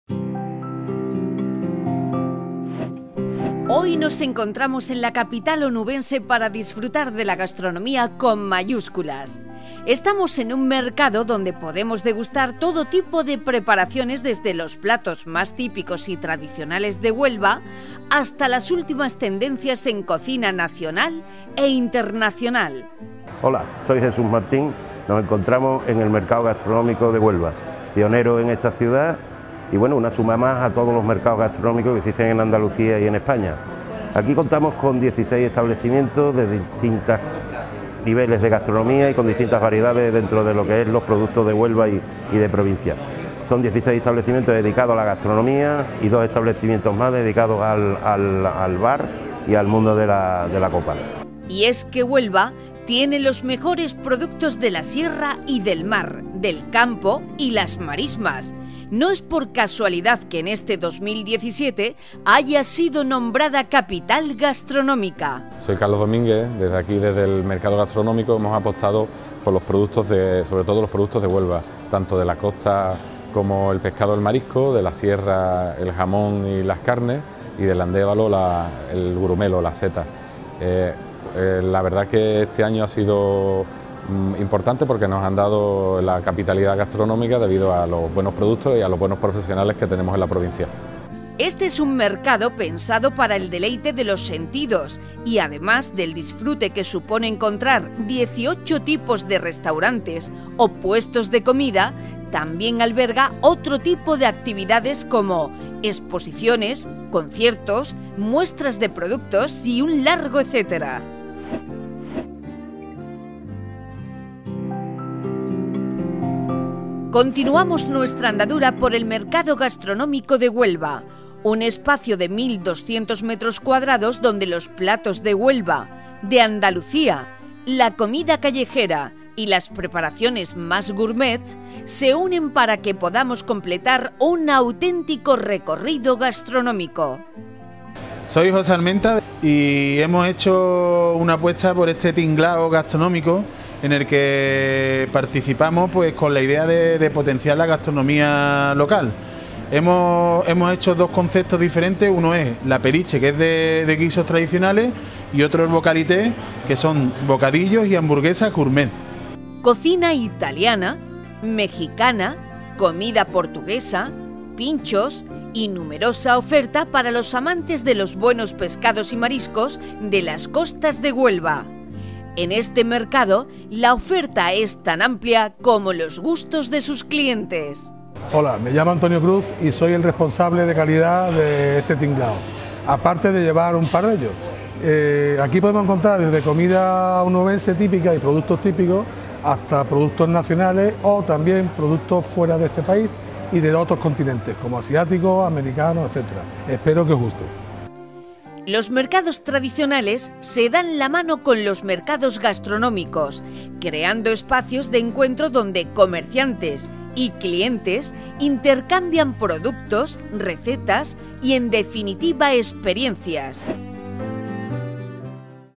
El Tinglao. Mercado gastronómico de Huelva
El Tinglao Gastronómico es un espacio ubicado en el Centro Comercial Aqualon de Huelva donde disfrutar de la cocina tanto local y nacional como internacional. Distribuido en 18 establecimientos, en él se pueden degustar especialidades como carnes a la brasa, croquetas, cocina italiana, crepes y dulces, jamón, marisco y pescaíto frito o cocina mexicana, entre otras.

Otros datos de interés:
El Tinglao Gastronómico